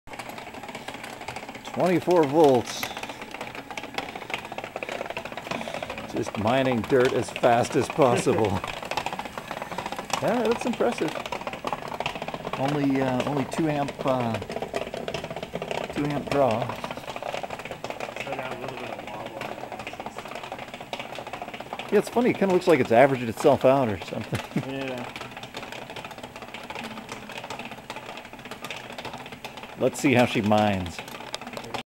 LunAlaska Robot Conveyor Test
Today's test run with a DynaVeyor conveyor belt, with angle aluminum digging claws attached.  We're using it in the LunAlaska project to excavate and transport lunar regolith (moon dust) simulant.